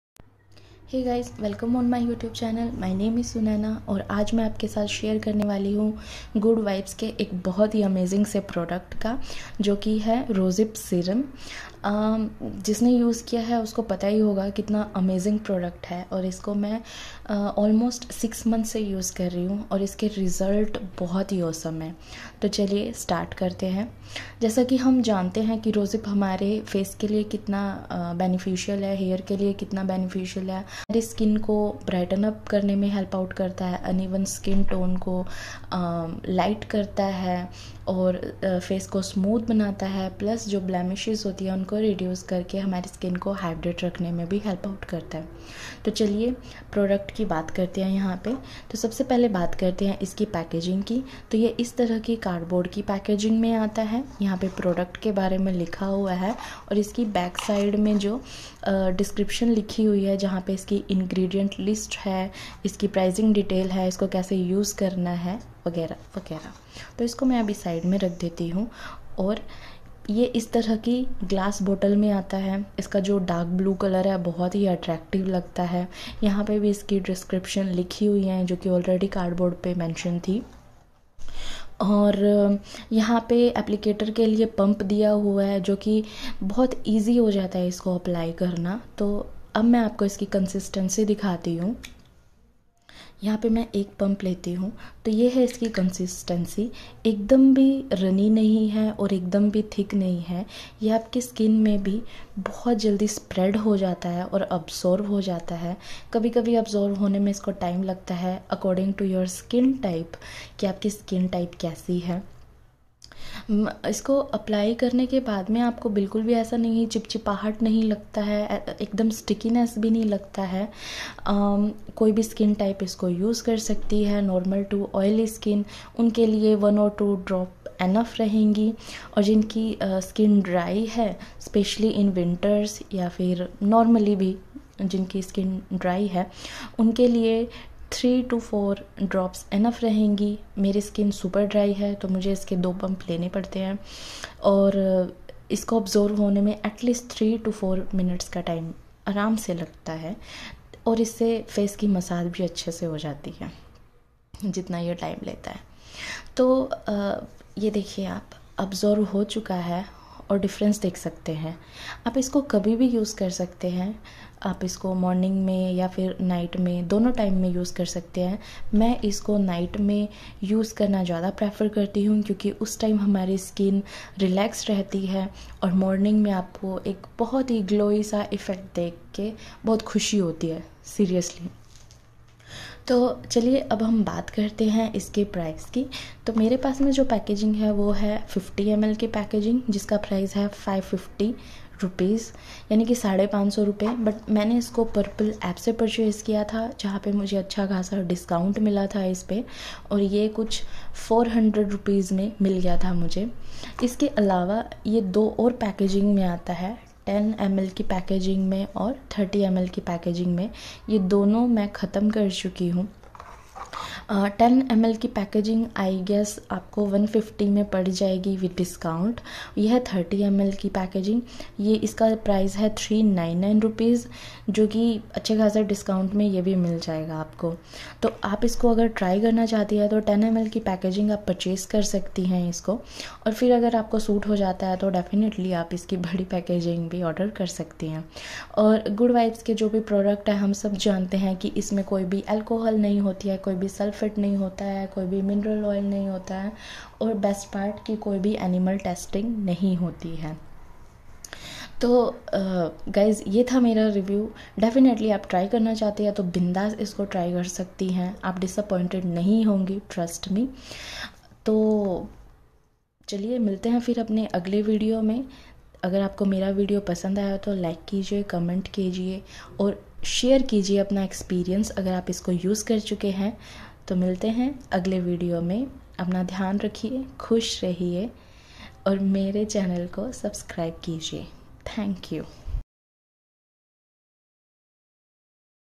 Good Vibes Rosehip Face Serum for Radiant Glow || Product Review
Hello beauties 🙋🏻‍♀️
This is my honest review about this product and also a non sponser video. This face serum infused with a goodness of Rosehip, enhance overall appearance of your skin and making it brighter and gives you a healthy, natural glow
Benefits:---
🔸 Keep your skin hydrated n soft.
🔸 Look healthy moisture and gives you glowing radiant look.
🔸 Lightweight, non greasy formula.
🔸 Bright uneven skin tone.
🔸 Reduce dark spots n wrinkles.
🔸Mild n beautiful fragrance.

Good Vibes products are chemical free like Sulphate, Parabens and cruelty free❤️

Thank youuu 🤗